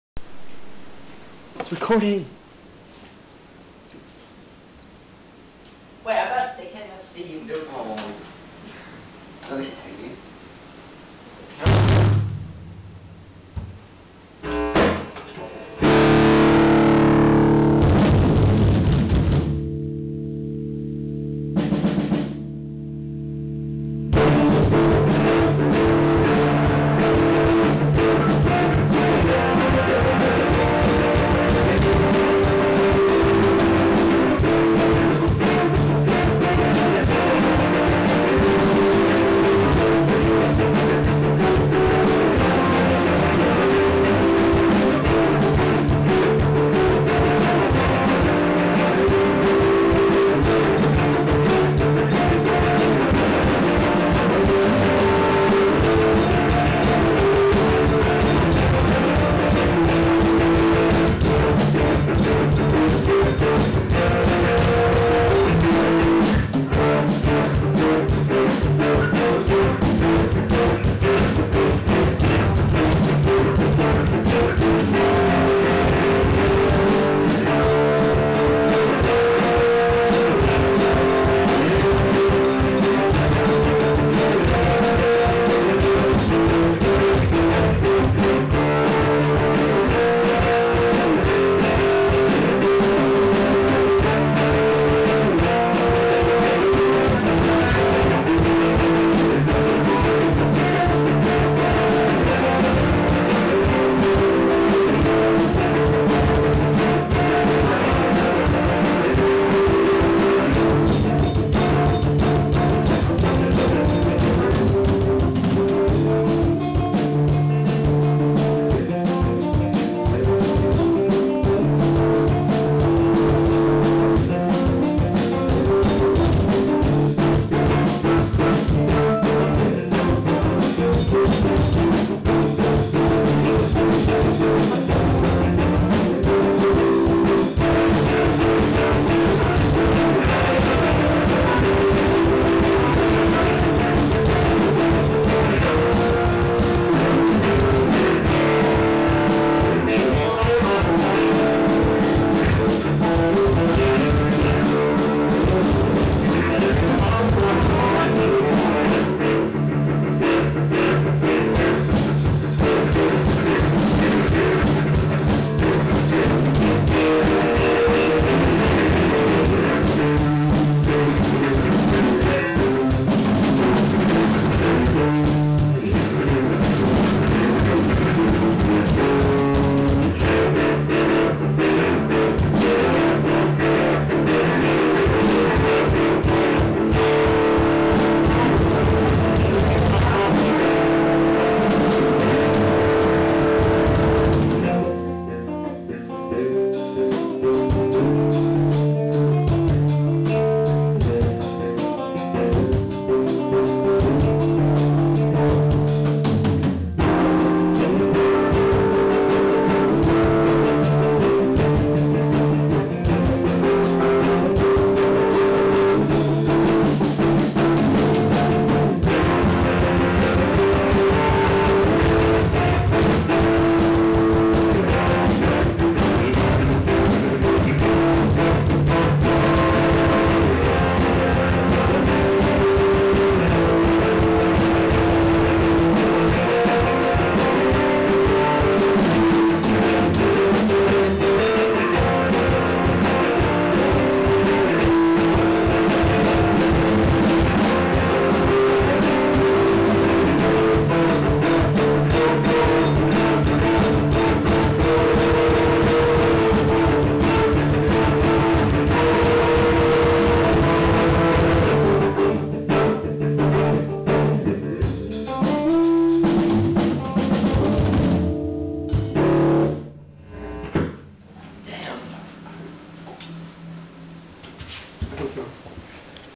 jason trevor jam
me and my bro trevor just practicing